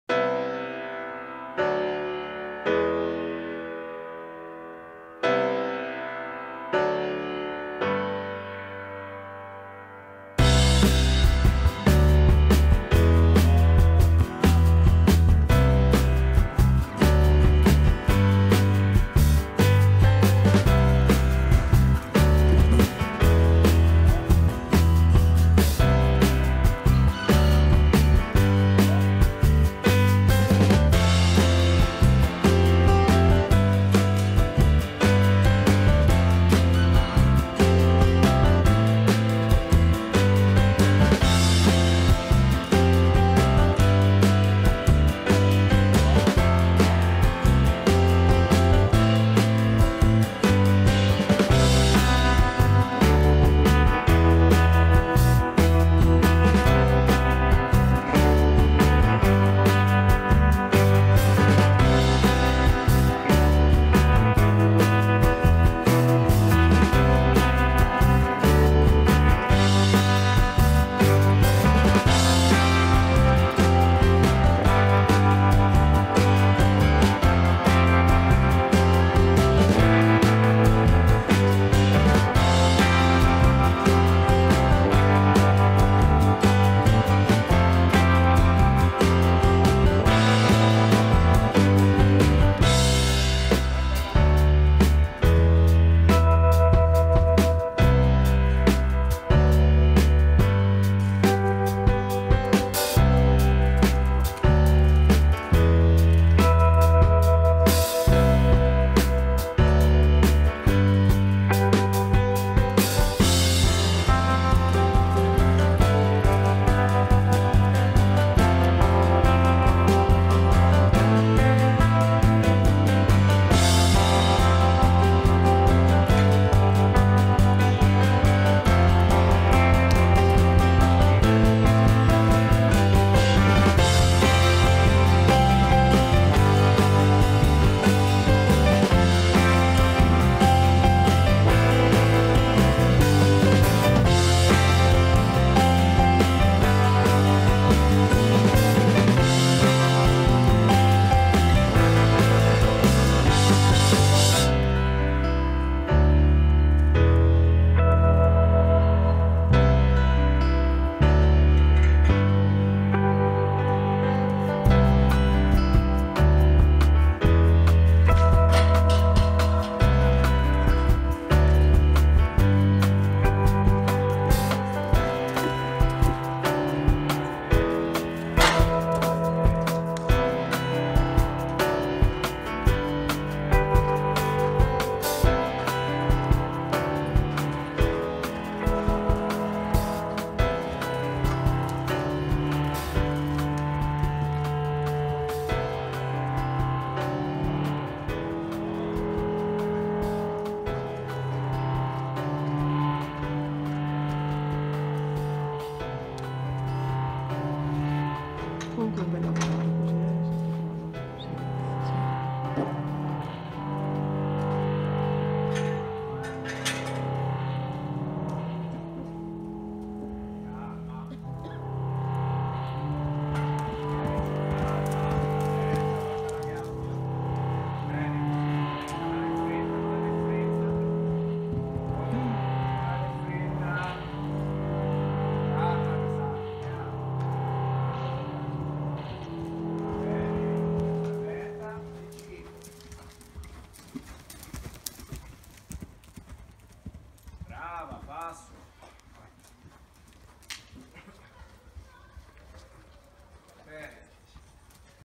STAGE DI COMPLETO CON EMILIANO PORTALE 22 GENNAIO 2017
Il giorno di salto ostacoli! Ecco alcuni dei giovani agonisti al lavoro il secondo giorno dello stage.